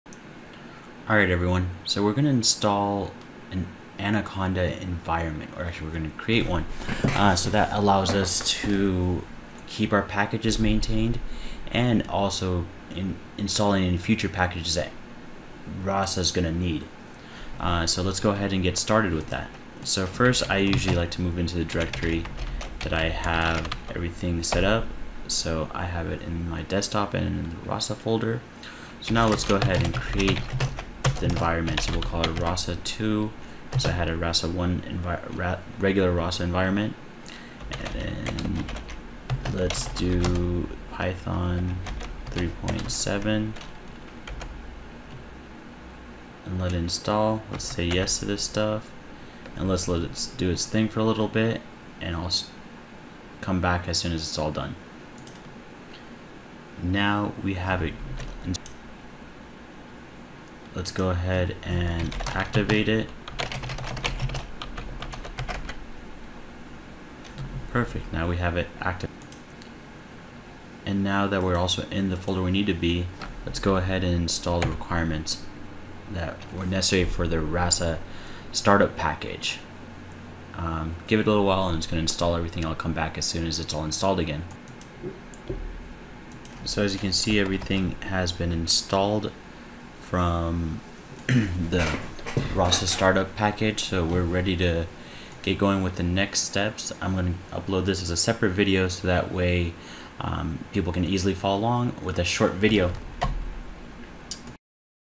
Rasa Chatbot - Anaconda Environment Setup - Windows 10
Here is how you can use Anconda on your Windows 10 systems to setup Python and Anaconda. I will go over how to you can setup your Anaconda environment, activate it, and then run the steps necessary for the start-up package available for the Rasa Platform.